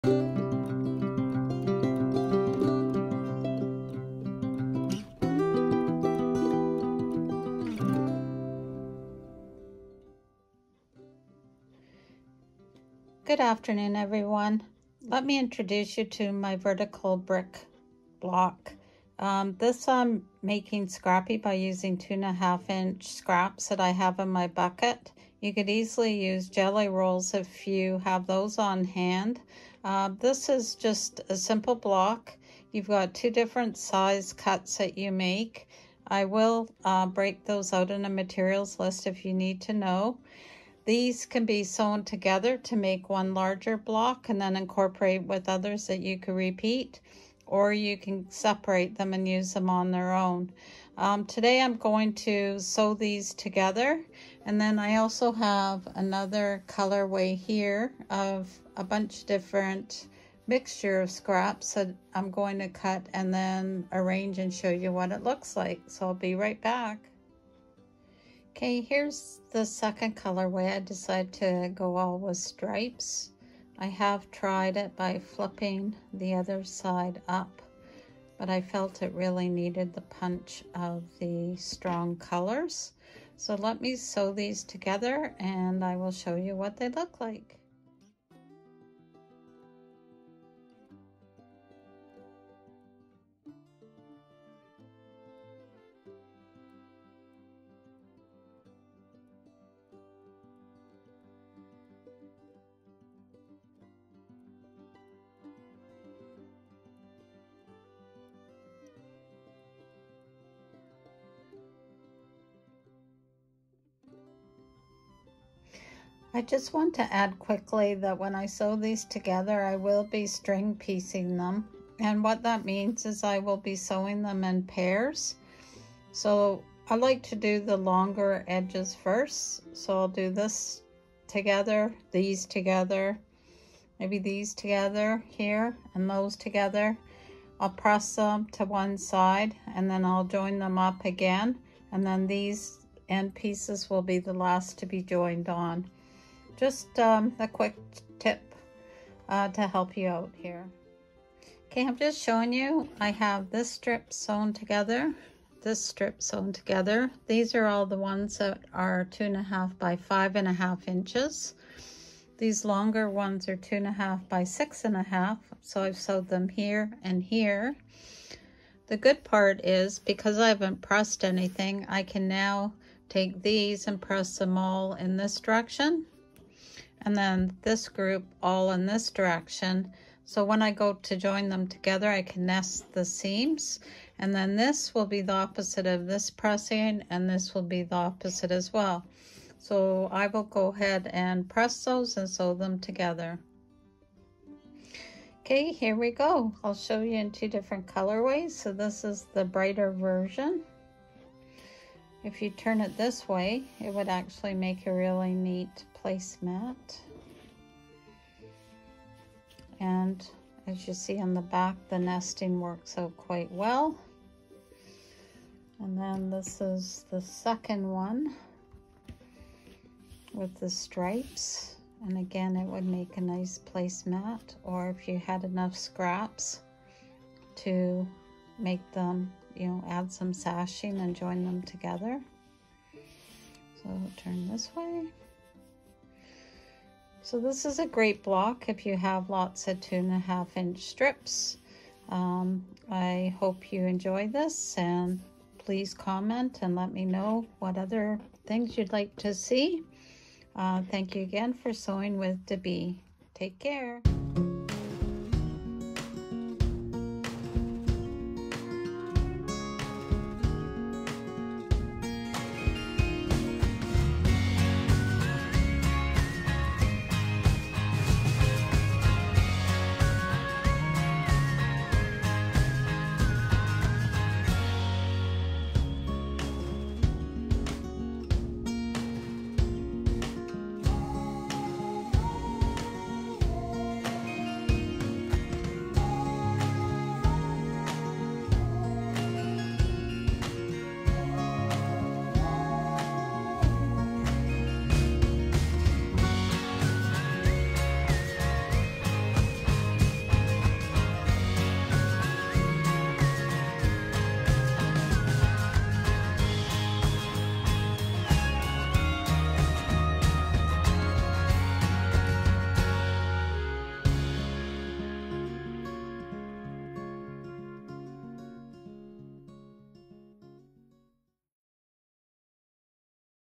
Episode 24: Vertical Brick Scrappy Block with Free Pattern
Goes together quickly using 2-1/2” strips or scraps. Another great block with visual impact that builds nesting skills. Use jellyroll strips or scraps or cut from fat quarters or yardage. Easy to make and an addictive block, that can be used for any size quilt or table runners or placemats, let’s just say it is so versatile.
Each block needs;
4 strips at 2-1/2 x 6-1/2” and
12 strips at 2-1/2 x 5-1/2”
See video middle and end for layout and color ideas.
As always Sew With De’bee